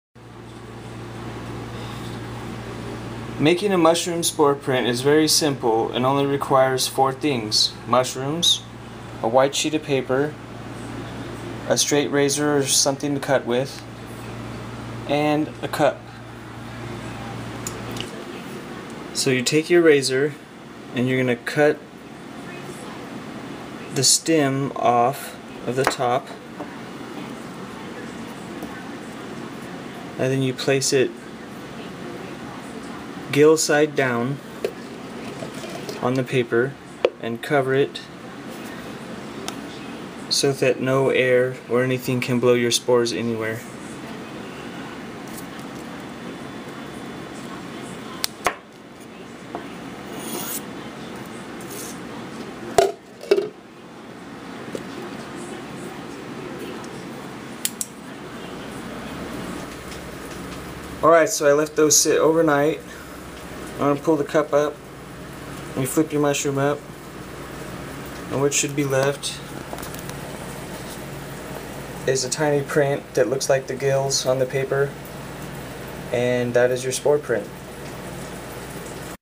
How to make a Mushroom Spore Print
Making a mushroom spore-print for Fun, Art, and Species Identification.  PLEASE NOTE:  DO NOT HANDLE UNKNOWN MUSHROOMS!!  THIS VIDEO IS INTENDED FOR USE WITH ALREADY VERIFIED NON-POISONOUS, EDIBLE MUSHROOMS!